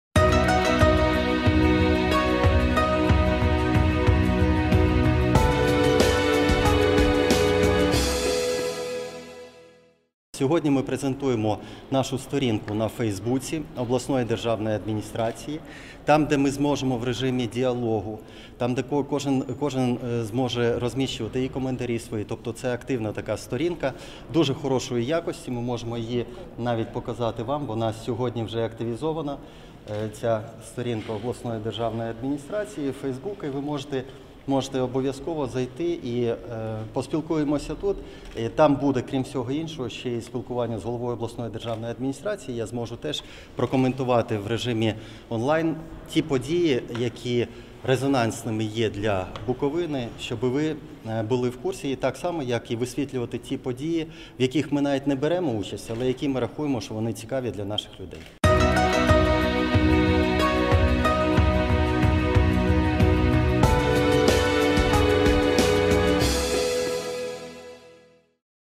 Чернівецька облдержадміністрація відтепер має сторінку у Facebook
На сьогоднішньому брифінгу Михайло Папієв зробив «міні-презинтацію» сторінки ОДА в соціальній мережі Facebook. Чиновник запевнив, що спілкування на сторінці буде вестися в режимі діалогу і кожний зможе залишати свої коментарі.